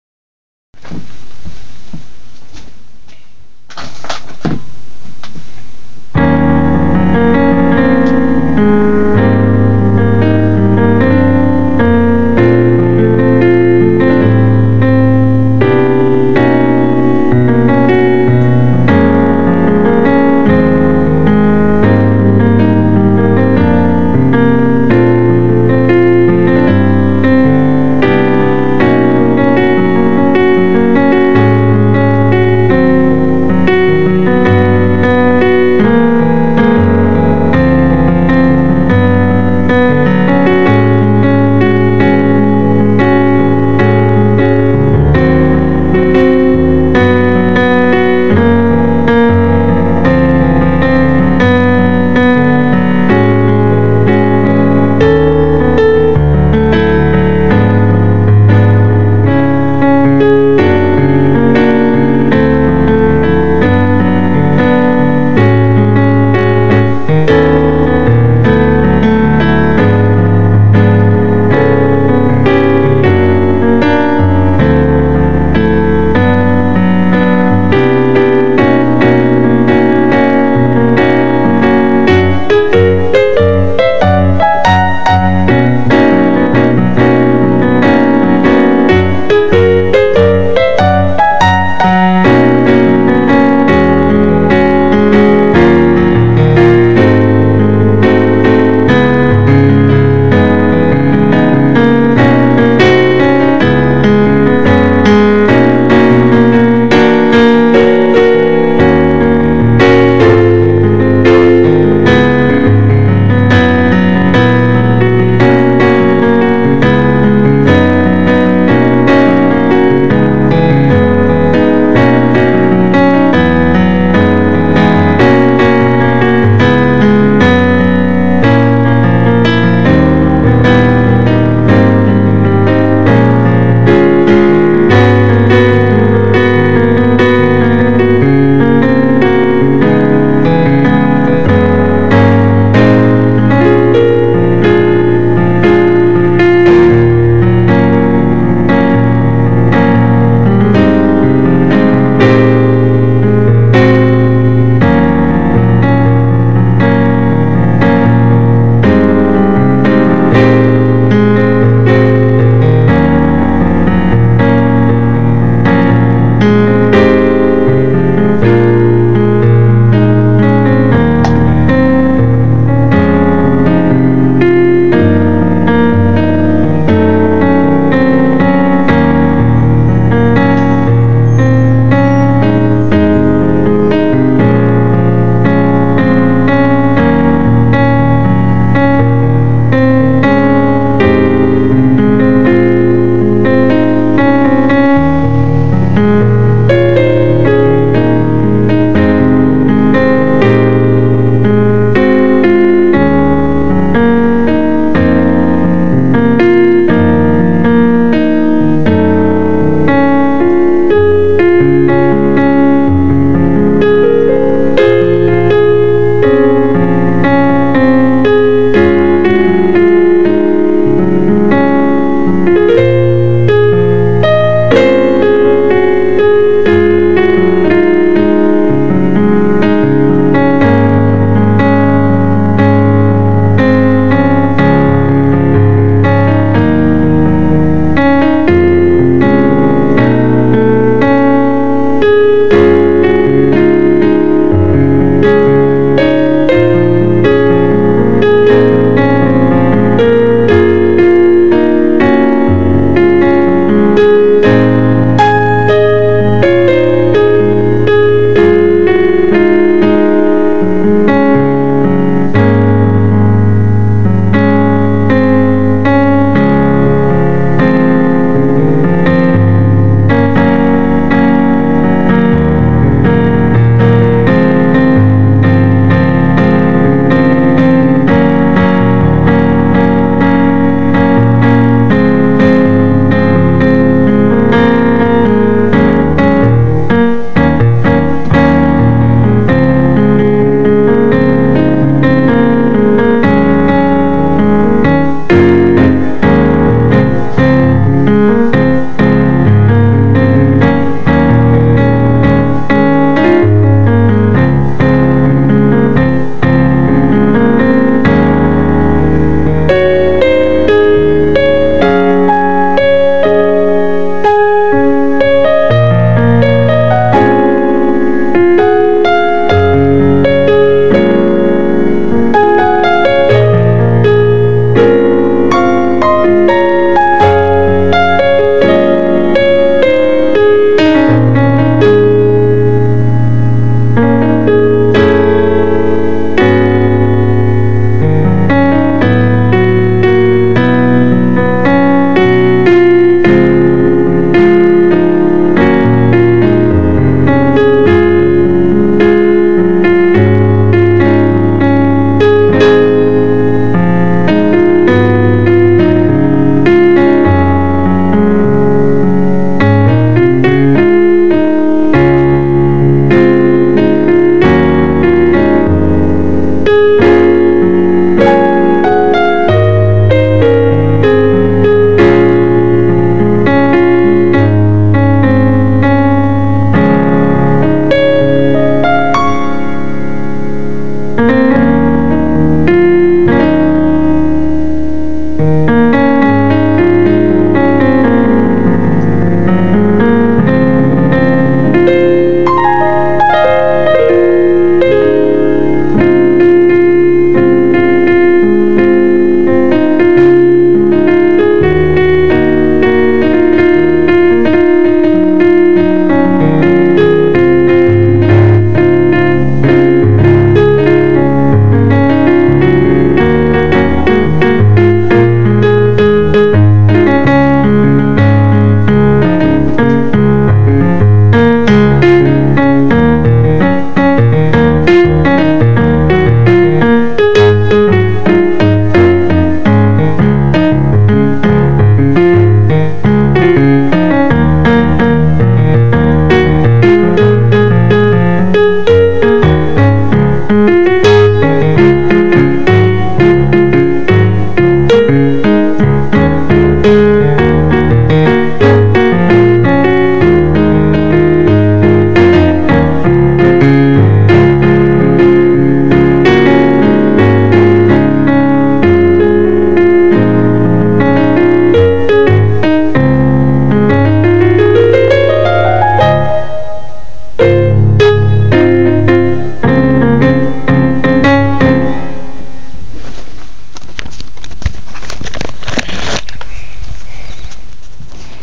Jurrell freestying on the keys.
My talented cuz doing one of the things he does best.  Check him out